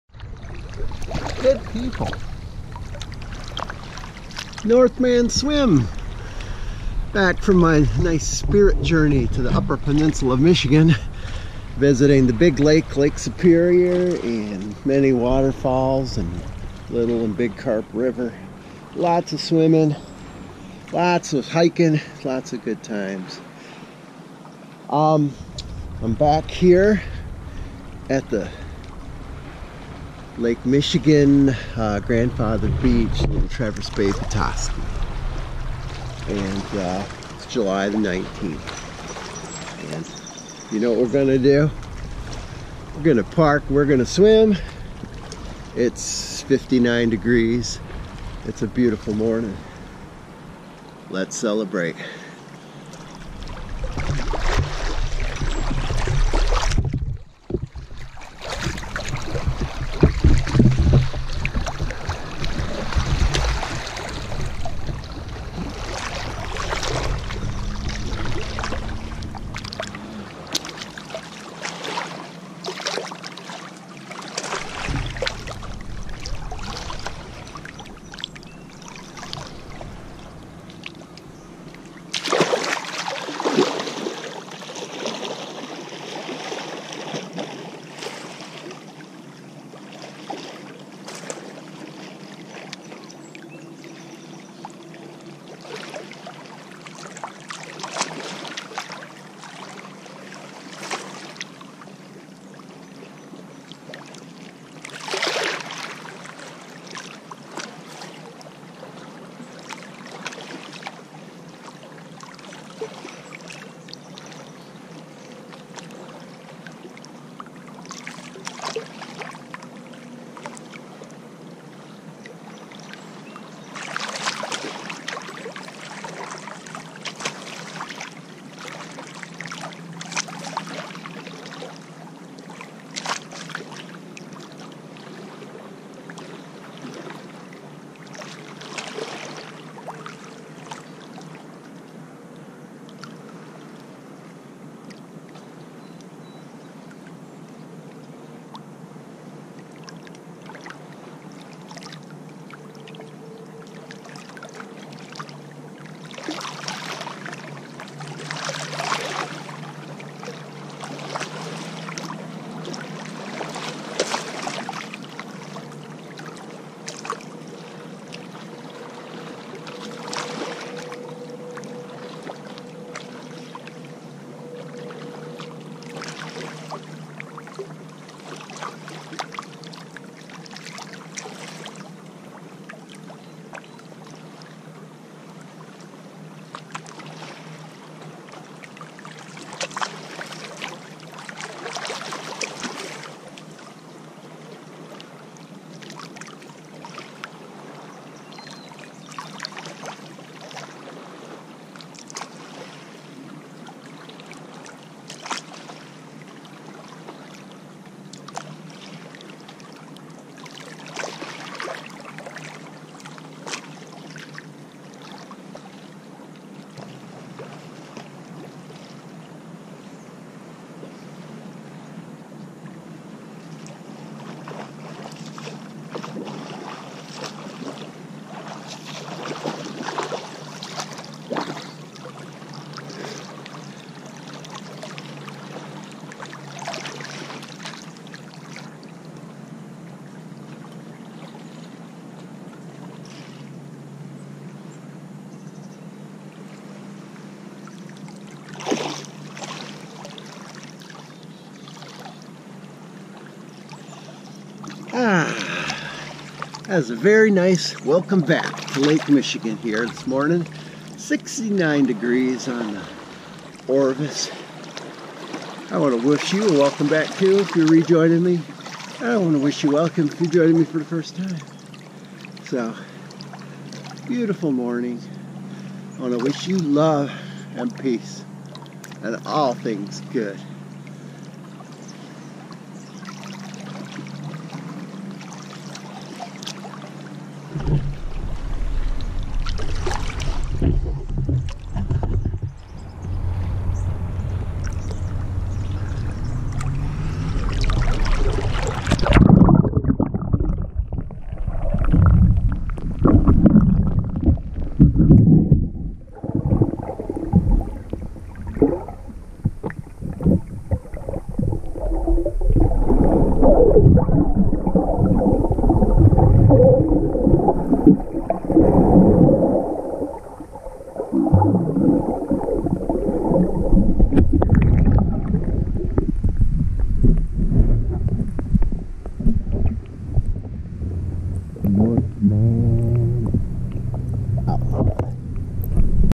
Svlog 207: Welcome Back
Northman returns to Grandfather Beach and has a duck visitor.  Ait 59 F Water 69 Total swim 24:35